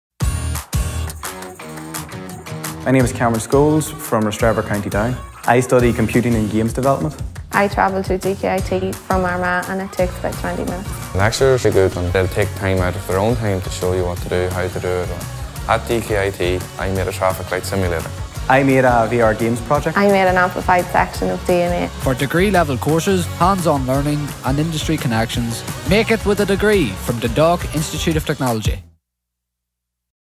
Make It at DkIT - Apply from Northern Ireland (NI)
At DkIT our students learn by doing. Our hands-on approach to education means you won’t be sat silently in the classroom all day long. Our lecturers bring lessons to life through real experiences in labs, in the work place, through live projects and practical group projects. Located just 20 minutes from Newry city, our state-of-the-art campus is so convenient for students from NI.

ThinkDkIT for hands-on learning, a supportive campus community and career-focused degree courses. Make something of yourself at DkIT.

Hand-On Learning / 45+ career-focused courses / Support for your future